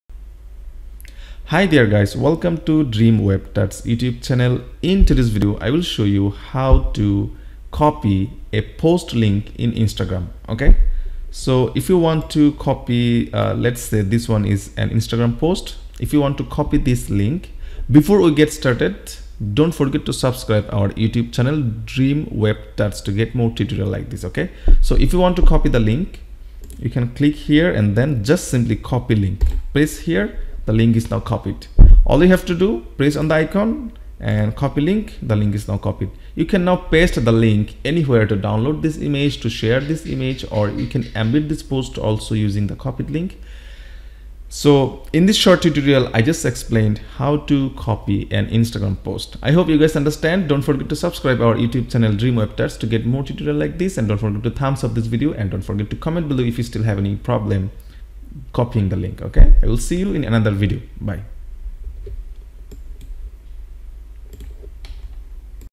How to Copy Instagram Link - Post, Image & Video
Guys, If you don't know yet how to copy an Instagram post's link, you can copy a link by watching this video. Here I have explained well how to copy Instagram post link. Hope you enjoyed this short tutorial.